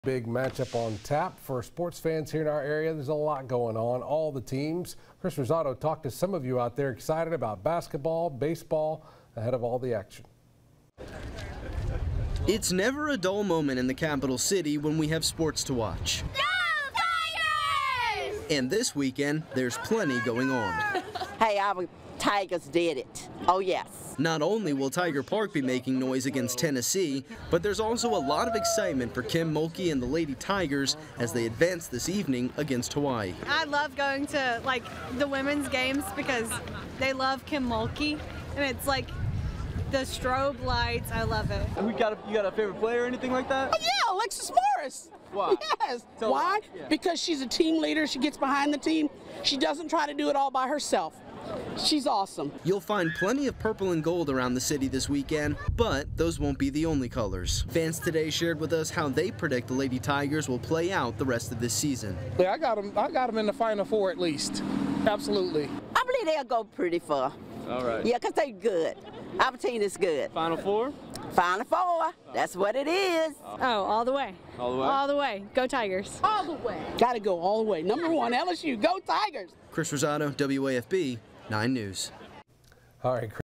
LSU fans react to the excitement surrounding the Tigers
LSU fans react to the women's basketball win over Hawai'i.